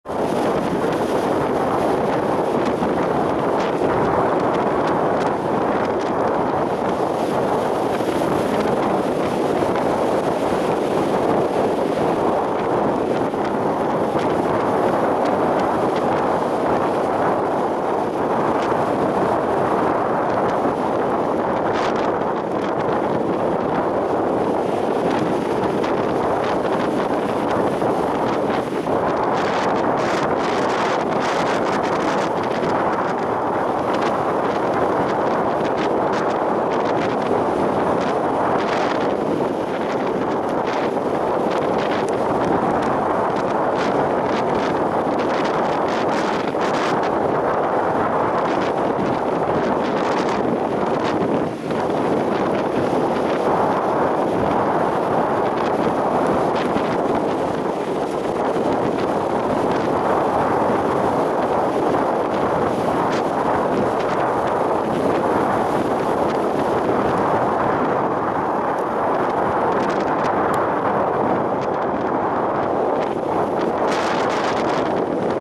Сёрфер летит над волнами в Зеленоградске
Ветер настолько сильный, что доска не прикасается к воде большую часть времени.